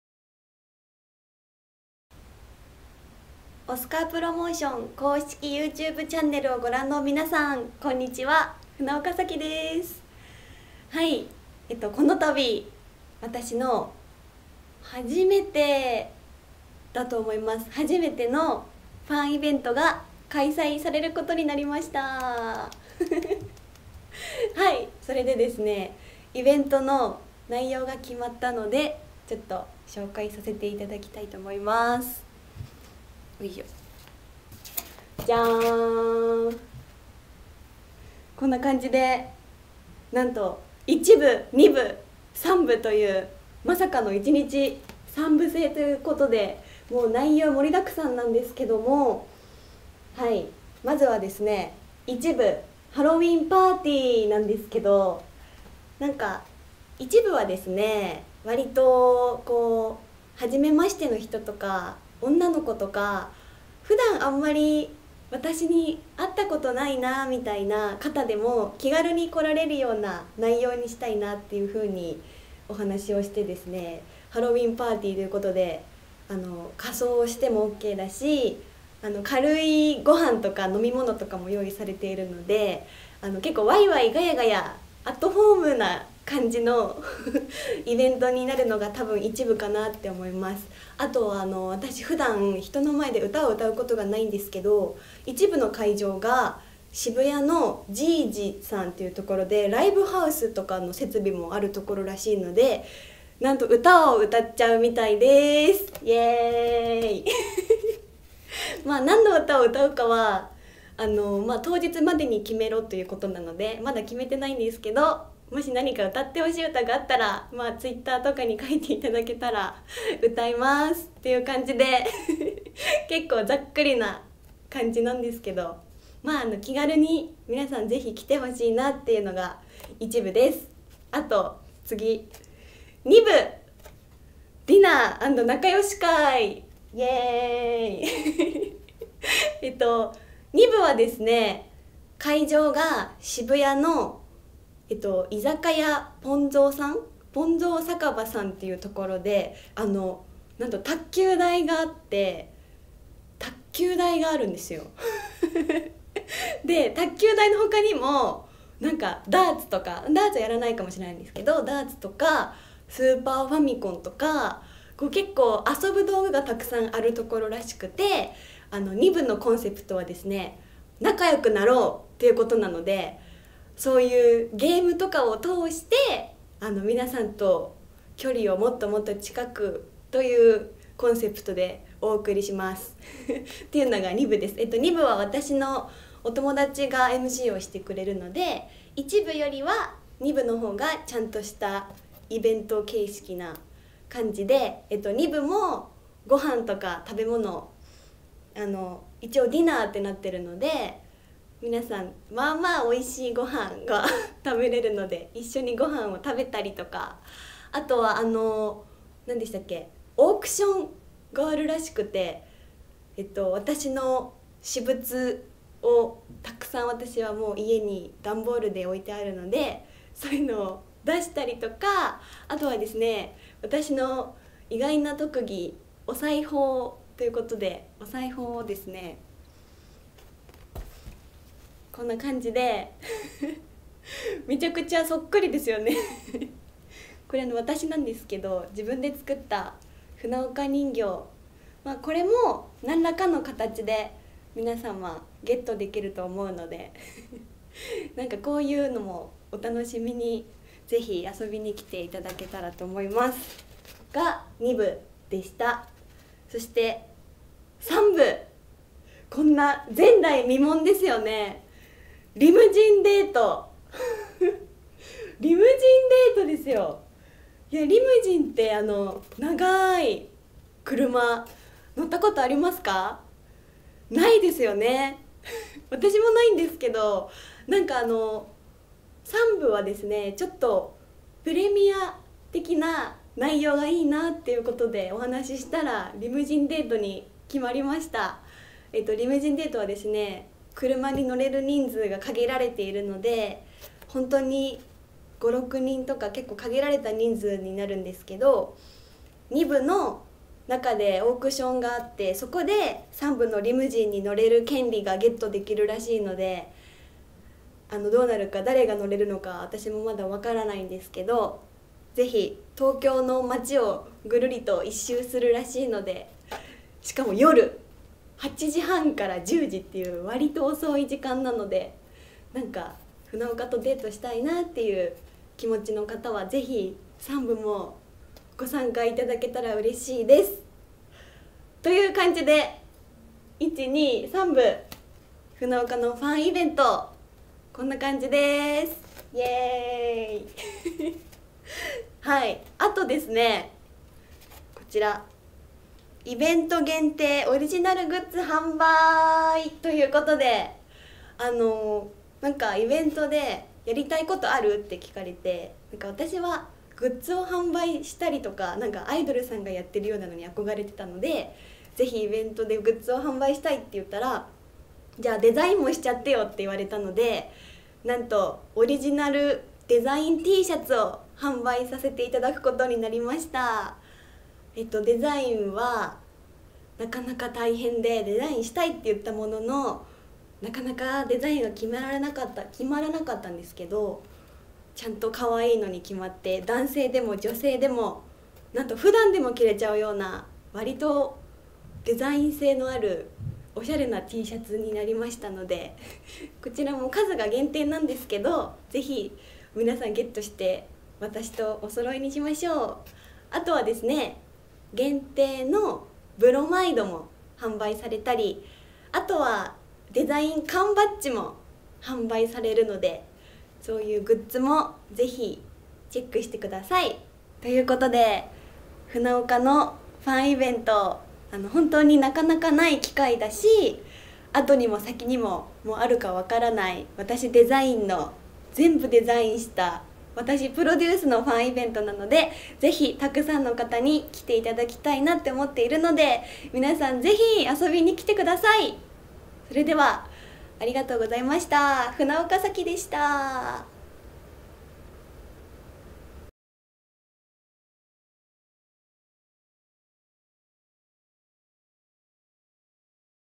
いよいよ10月22日(土)開催！船岡 咲のファンイベントインタビュー！
〜船岡とみんなのキズナ会〜

■第1部■
船岡とハロウィンパーティーハロウィンくまアイス
時間：13:00〜15:00(予定)
前売料金：8000円
＊フリーハンドドリンク＋軽食付き
＊船岡ハンドメイドのオマケ①プレゼント
＊2ショットトーク&スマホ撮影タイムあり
＊仲良しチェキ撮影タイム(料金別¥1,000)

■第2部■
船岡とディナー&ゲームで仲良し会虹トロピカルカクテル
時間：17:00〜20:00(予定)
@渋谷・卓球酒場ぽん蔵

前売料金：10,000円
＊フリーソフトドリンク＋ディナーメニュー
＊船岡ハンドメイドのオマケ②プレゼント
＊愛のピンポンラリー大会
＊ファンタスティックオークション

■第3部■
人数限定！超プレミア企画
豪華リムジンでTOKYO NIGHTボトルハート

時間：20時半開始(予定)     ※22時頃終了予定
◎第3部参加券は、第2部のオークション内で販売します。